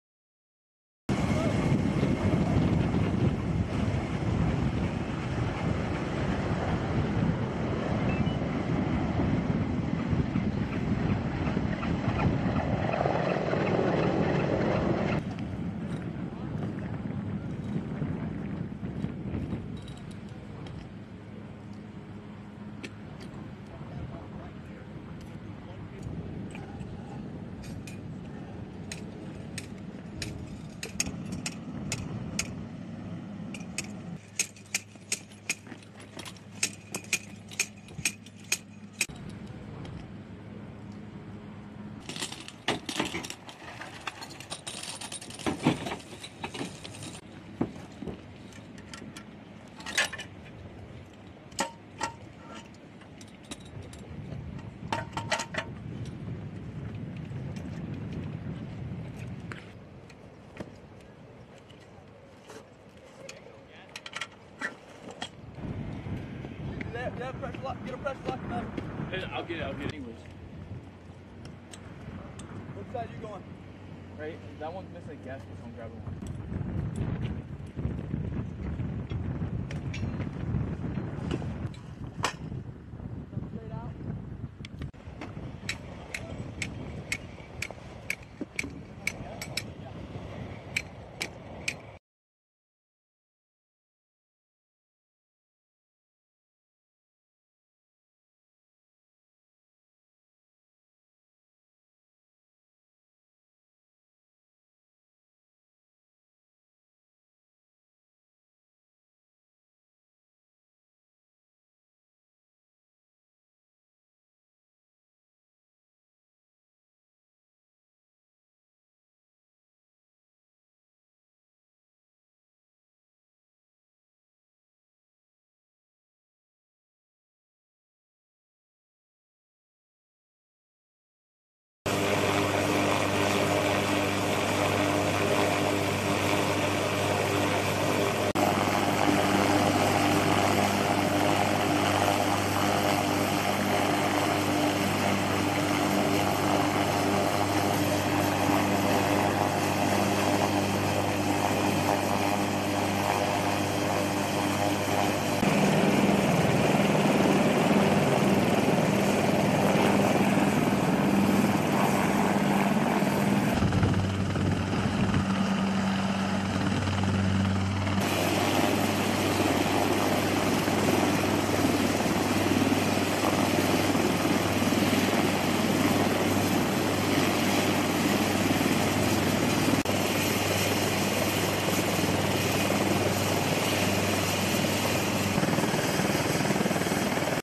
MAG-24 Expeditionary Advanced Base (FARP) Operations
MAG-24 Expeditionary Advanced Base Operations
HI, UNITED STATES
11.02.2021

U.S. Marines with Marine Wing Support Squadron (MWSS) 174, perform a simulated forward arming and refueling point (FARP) at Pacific Missile Range Facility Barking Sands, Hawaii, Nov. 2, 2021. MWSS-174 conducted this training to demonstrate readiness and combat proficiency in austere, expeditionary evironments within the Indo-Pacific. (U.S. Marine Corps video by Cpl. Dalton J. Payne)

Film Credits: Video by Cpl. Dalton Payne
1st Marine Aircraft Wing